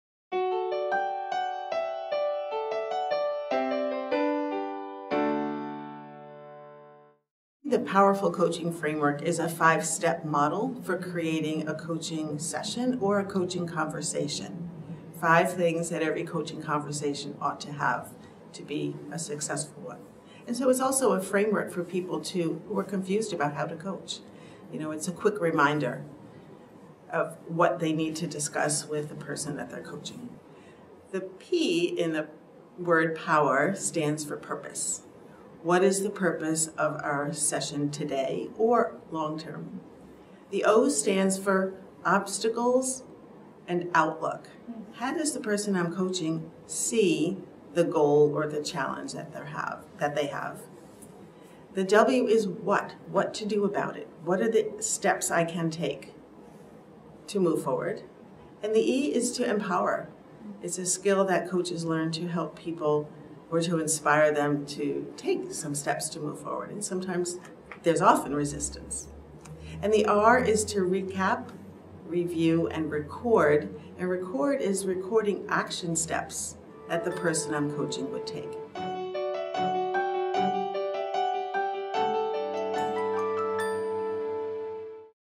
Five POWER Keys to Coaching
The five keys to coaching, Purpose, Obstacles, What, Empower,  and Review & Record are introduced.